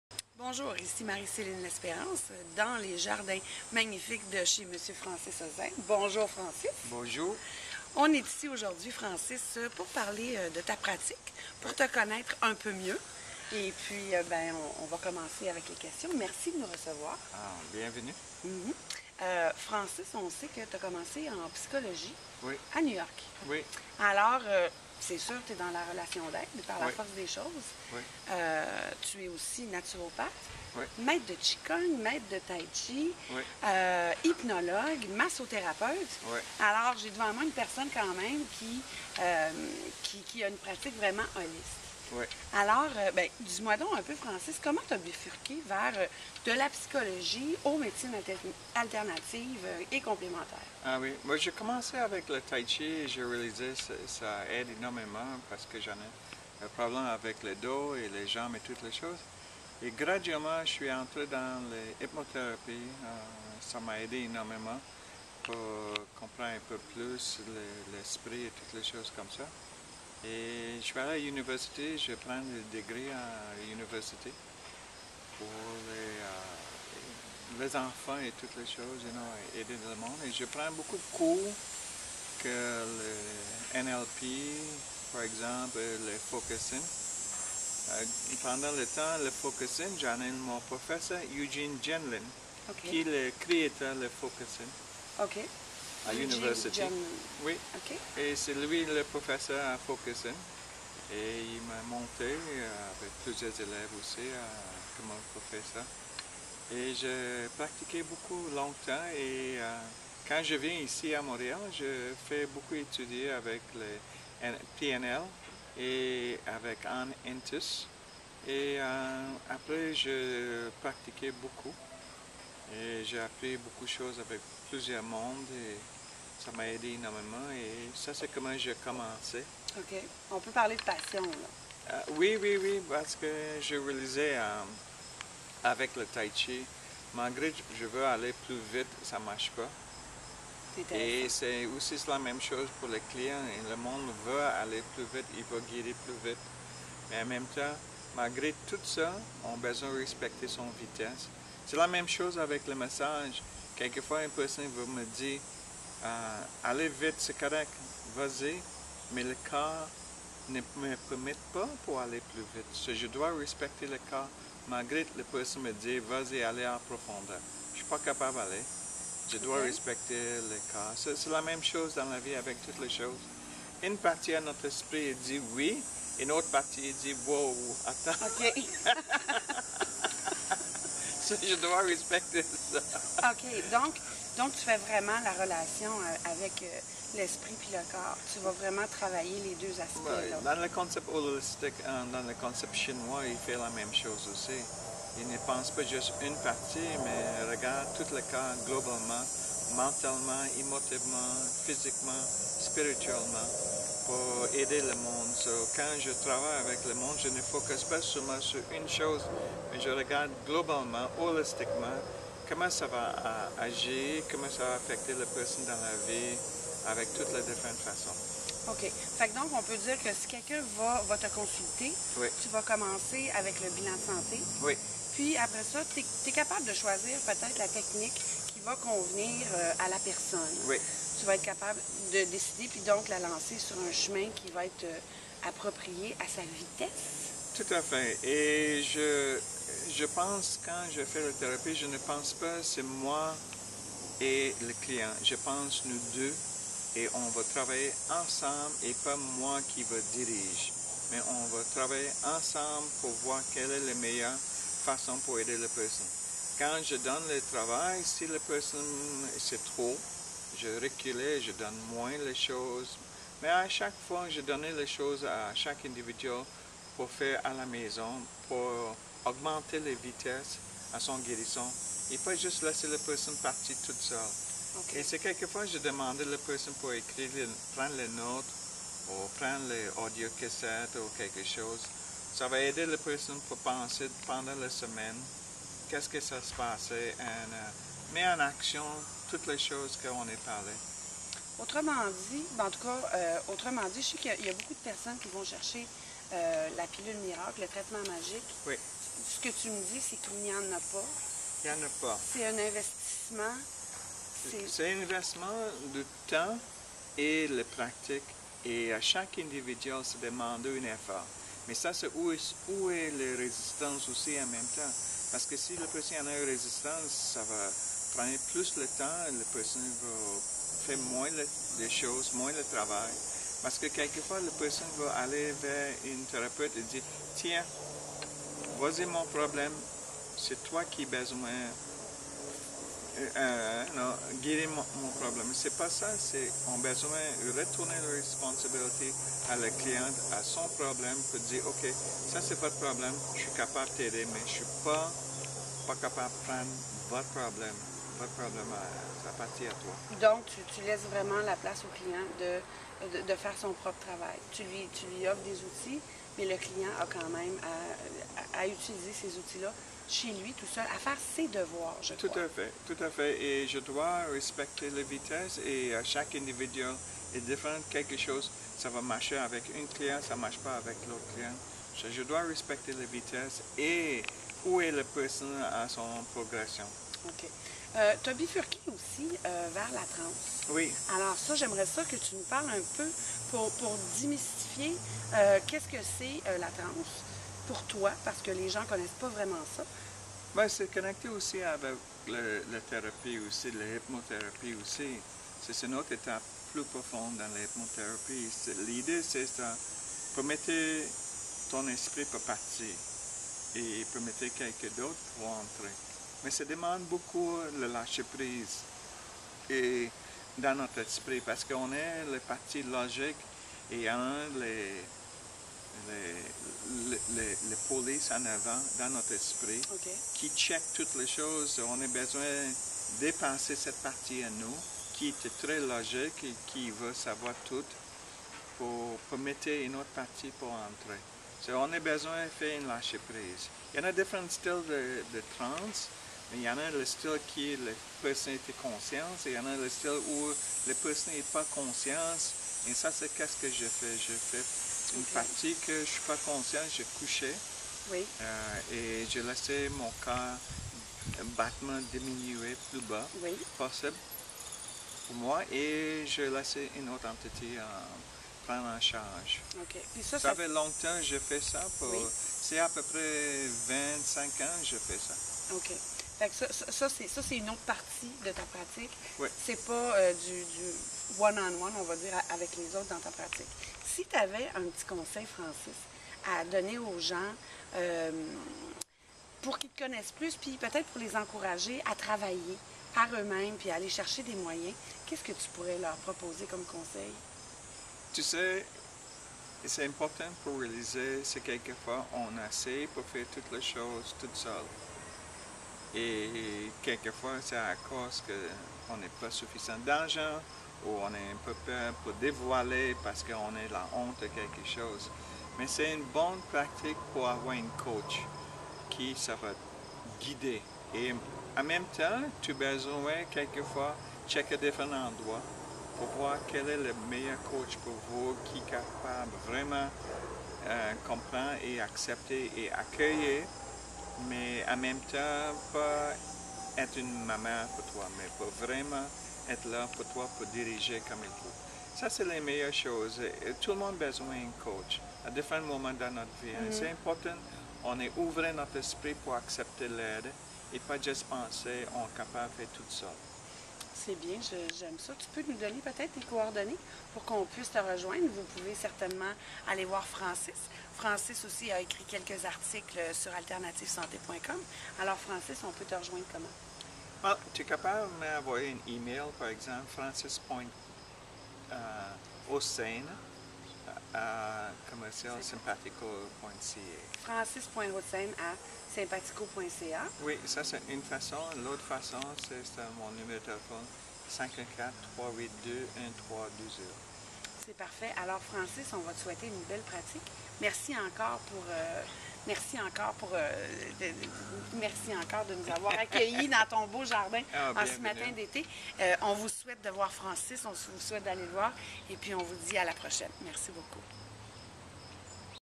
Francis Hossein, naturopathe, taï-chi coach, hypnothérapeute
AlternativeSanté.com vous présente Francis Hossein, naturopathe, hypnothérapeute, taï-chi coach et médium. Francis est un diplômé en naturopathie d'une école naturopathique de New-York. Il exerce dans le domaine des alternatives complémentaires santé depuis plus de 20 ans. Il reçoit ses patients à partir de sa clinique naturopathique à Montréal quartier Ahuntsic... Ses dans son jardin que fut réalisée l'interview qui a été perturbée quelques instants par un bruit dérangeant mais que nous avons pu surmonter, espérant que vous ferz de même! Francis est un anglophone que les canadiens-français n'auront pas de difficulté à comprendre, ce qui ne sera pas évident pour les francophones d'ailleurs. Francis Hussein explique en détail dans cette vidéo son approche professionnelle avec les patients qui le consultent pour toute sorte de problèmes de santé dont certains ne semblent pas vouloir guérir avec le médecine conventionnelle. L'auto-guérison et son travail de coach en santé sont deux de ses missions professionnelles.